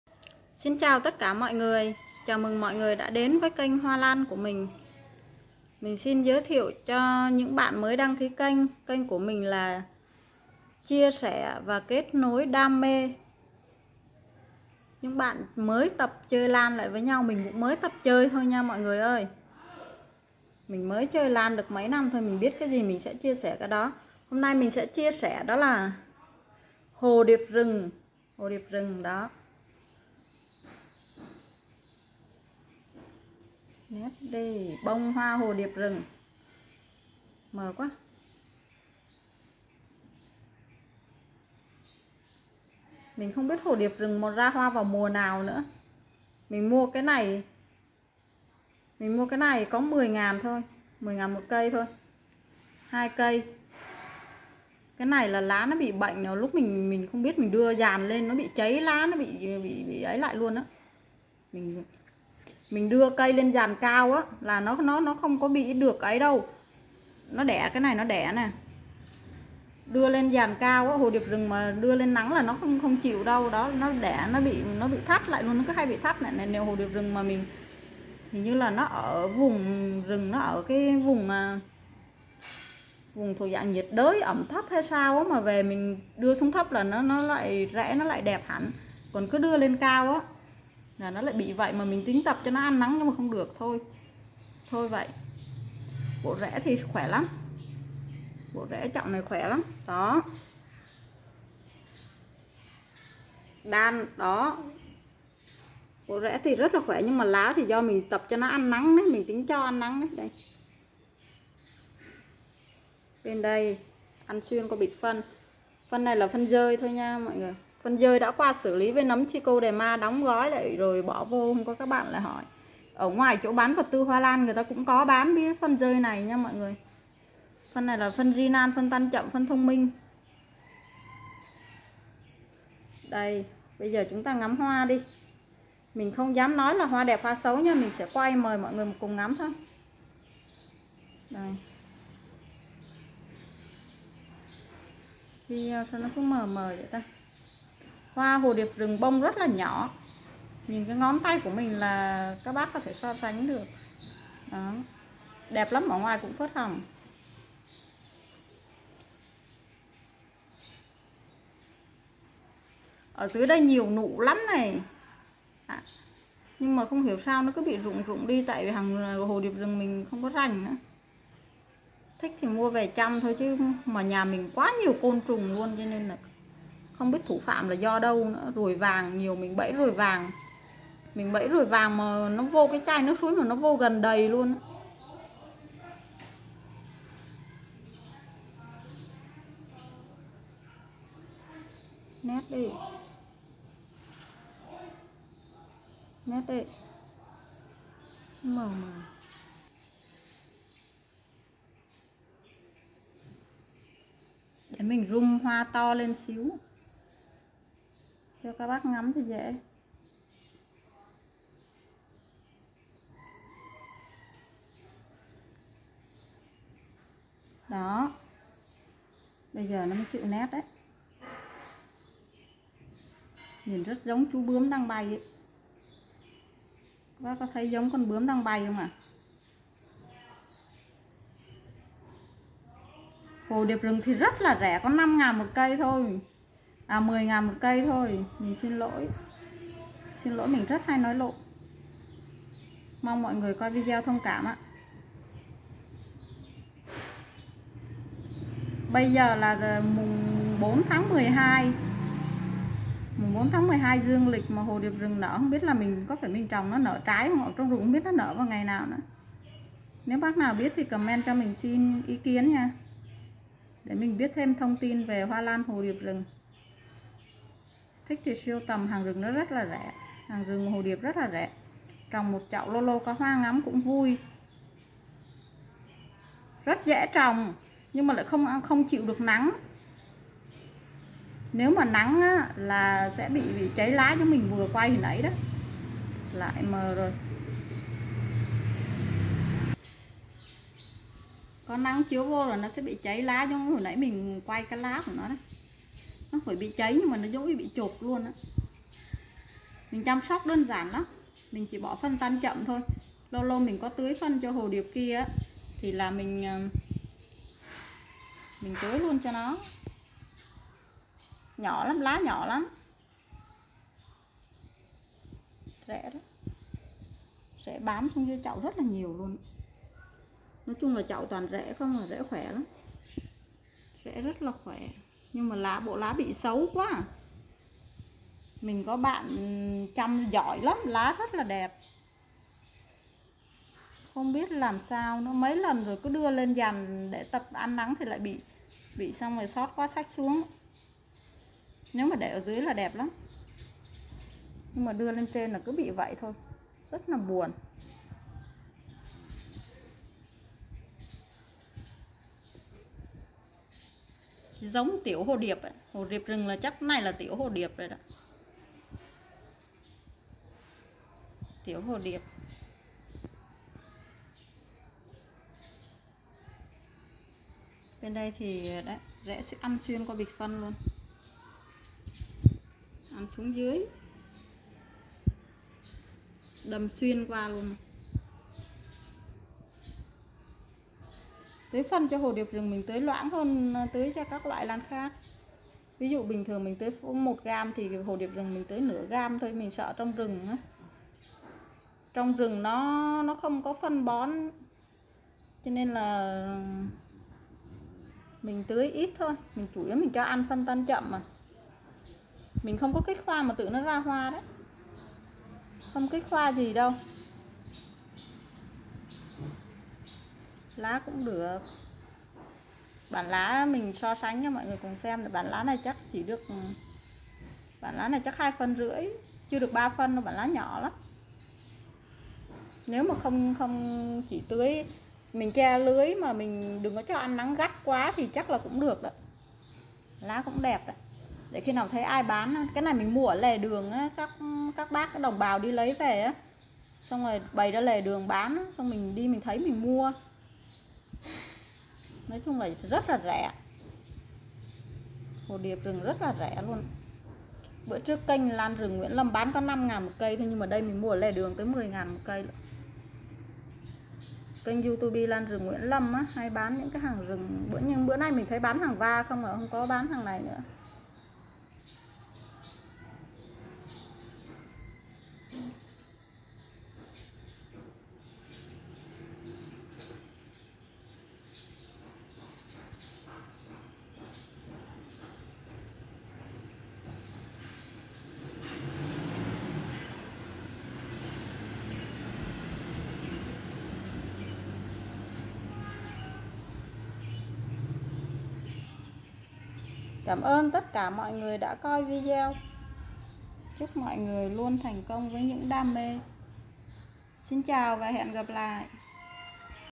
Hồ điệp rừng
Hồ điệp rừng cây nhỏ chăm sóc cũng như hồ điệp nhà không ưu ánh nắng cao. Hoa cũng nhỏ mùa sắc tạo nên vẻ đẹp riêng của nó. Hoa bền lâu tàn. rất đáng để siêu tầm. chúc cả sức khỏe và an nhiên. Cảm ơn tất cả mọi người đã xem video hoa lan hồ điệp rừng của mình.
video thuộc bạn quyền của Hoa lan Tuyến Hà.